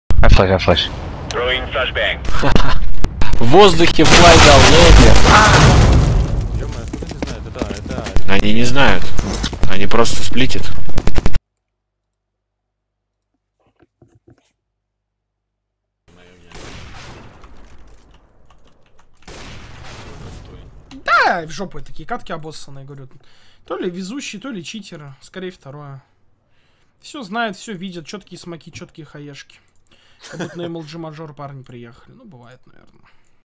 Попались в ММ против Cheatbanned. Результат.
Сыграли cobblestone против CB.